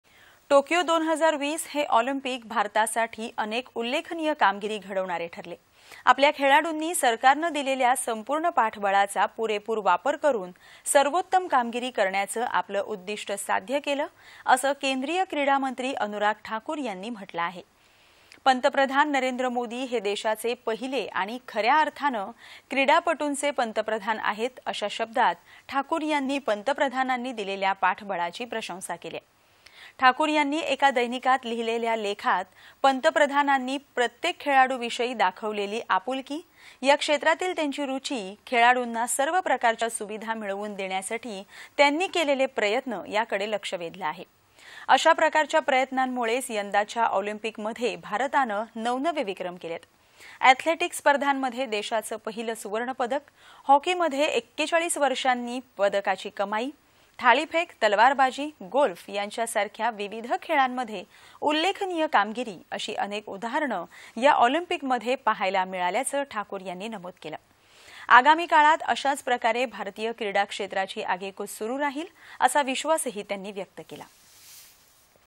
पंतप्रधान नरेंद्र मोदी हे देशाचे पहिले आणि खऱ्या अर्थाने क्रीडापटूंचे पंतप्रधान आहेत-अनुराग ठाकूर
२१.०८.२०२१ :
टोक्यो 2020 हे ऑलिंपिक भारतासाठी अनेक उल्लेखनीय कामगिरी घडवणारे ठरले.आपल्या खेळाडूंनी सरकारनं दिलेल्या संपूर्ण पाठबळाचा पुरेपूर वापर करून सर्वोत्तम कामगिरी करण्याचं आपलं उद्दिष्ट साध्य केलं, असं केंद्रीय क्रीडामंत्री अनुराग ठाकूर यांनी म्हटलं आहे. पंतप्रधान नरेंद्र मोदी हे देशाचे पहिले आणि खऱ्या अर्थाने क्रीडापटूंचे पंतप्रधान आहेत, अशा शब्दात ठाकूर यांनी पंतप्रधानांनी दिलेल्या पाठबळाची प्रशंसा केली आहे. ठाकूर यांनी  एका दैनिकात लिहिलेल्या लेखात पंतप्रधानांनी प्रत्येक  खेळाडूविषयी दाखवलेली आपुलकी, या क्षेत्रातील त्यांची रुची, खेळाडूंना सर्व प्रकारच्या सुविधा मिळवून देण्यासाठी त्यांनी केलेले प्रयत्न याकडे लक्ष वेधलं आहे. अशा प्रकारच्या प्रयत्नांमुळेच यंदाच्या  ऑलिंपिकमध्ये भारतानं नवनवे विक्रम केले. ऍथलेटिक्स स्पर्धांमध्ये देशाचं पहिलं सुवर्णपदक, हॉकीमध्ये 41 वर्षांनी पदकाची कमाई, थाळीफेक, तलवारबाजी, गोल्फ यांसारख्या विविध खेळांमध्ये उल्लेखनीय कामगिरी अशी अनेक उदाहरणं या ऑलिंपिकमध्ये पाहायला मिळाल्याचं ठाकूर यांनी नमूद केलं आहे. आगामी काळात अशाच प्रकारे भारतीय क्रीडा क्षेत्राची आगेकूच सुरू राहील, असा विश्वास त्यांनी व्यक्त केला आहे.